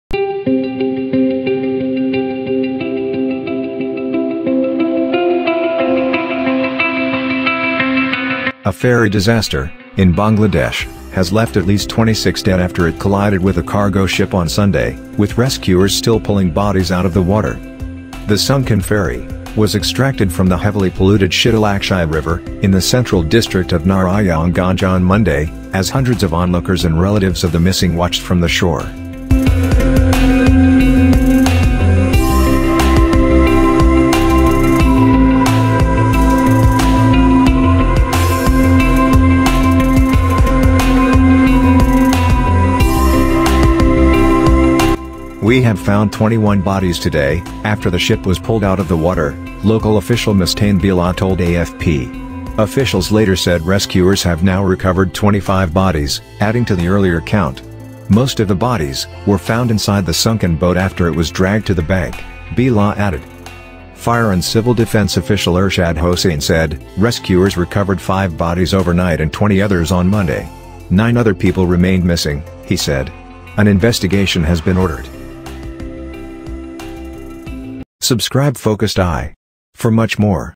Bangladeshi ferry disaster leaves 26 dead after colliding with a cargo ship.
The sunken ferry was extracted from Shitalakshya River on Monday after it crashed and capsized on Sunday
Officials said they have so far extracted 25 bodies from the boat, that was believed to be carrying at least 46
Some people were able to swim ashore to safety from the sinking one-and-a-half storey Sabit Al Hasan
Relatives gathered at the river bank to search for their loved ones. Rescue efforts by divers continued Monday
The boat was packed with people rushing to leave the city after the government announced a week-long nationwide lockdown from Monday to tackle a spike in COVID-19 cases